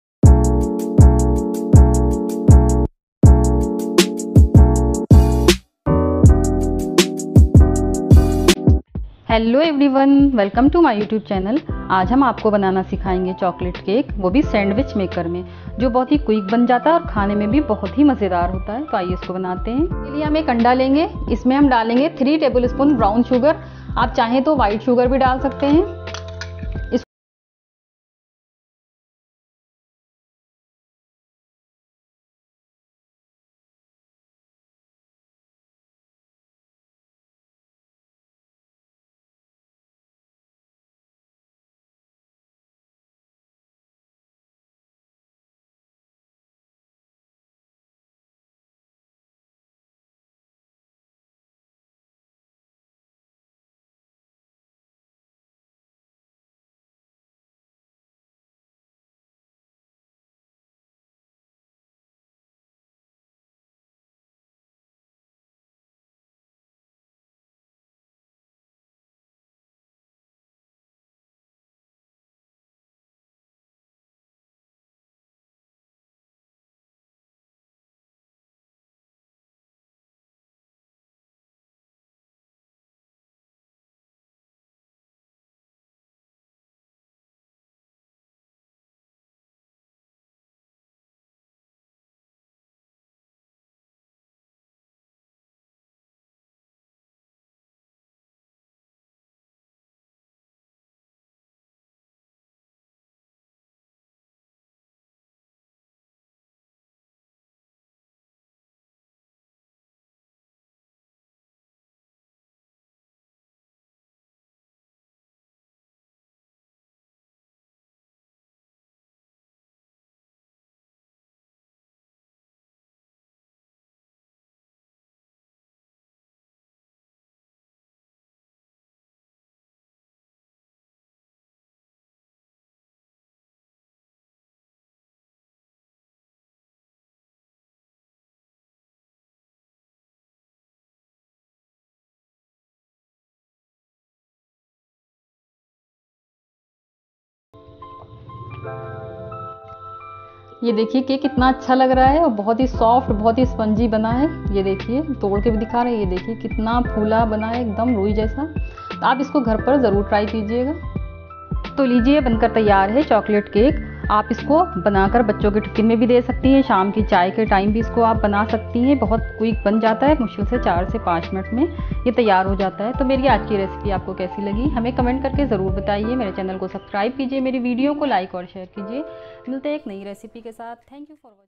QUICK RECIPE - CHOCOLATE TEA CAKE IN 5 MINUTES | अब चॉकलेट केक बनाए सैंडविच मेकर मे
For ingredients
1 egg
3 tbs brown sugar or white sugar
1/4 cup oil or butter
1/2 cup maida
1 1/2 tsp cocopowder
1/2 tsp baking powder
2 tbs milk
1 tsp vanilla essence
1 tbs choco chip